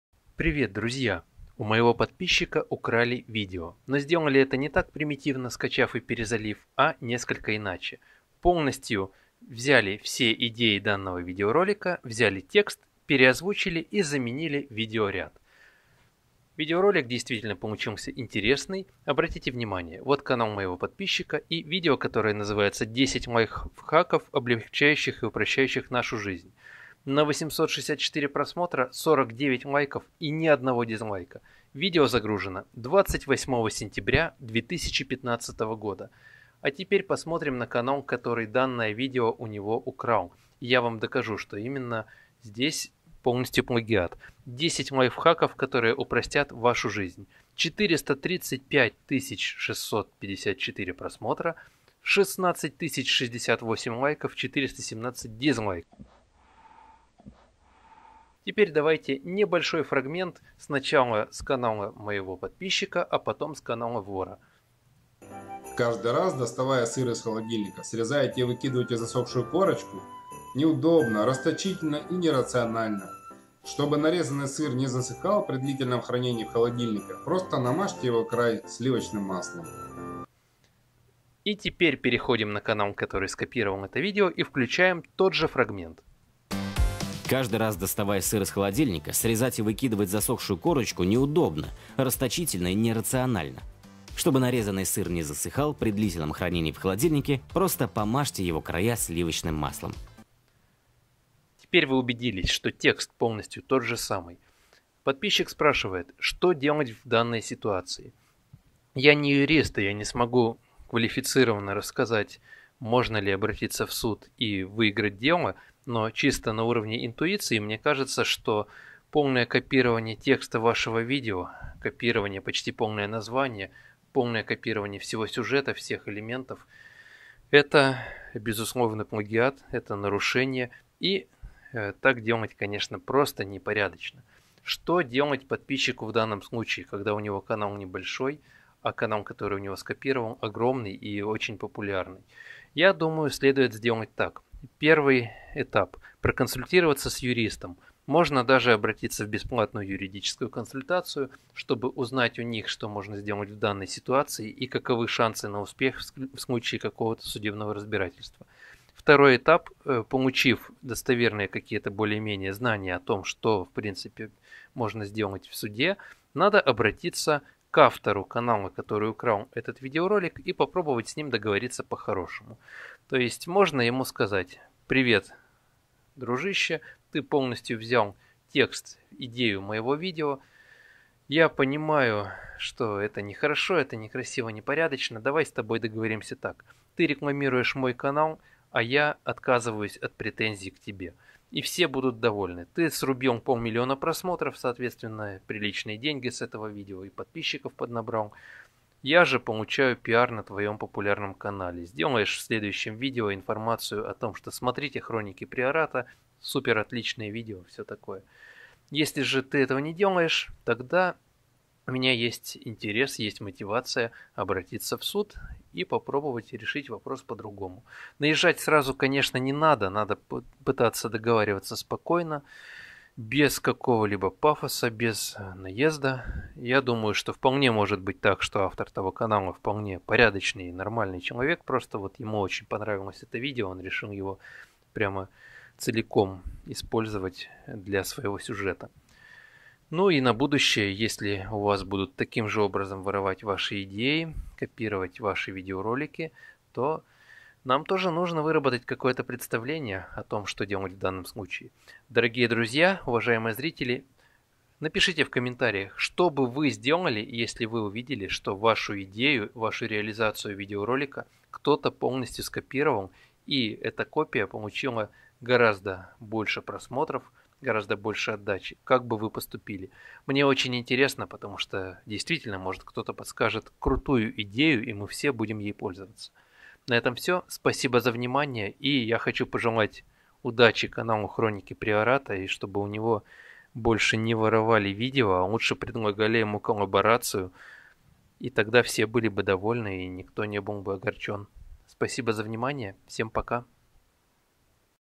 Украли идею и реализацию видео! Что делать?
Но не просто перезалил, а заменил видеоряд и зачитал текст своим голосом. Видео получилось очень успешным - полмиллиона просмотров. Что делать в данной ситуации?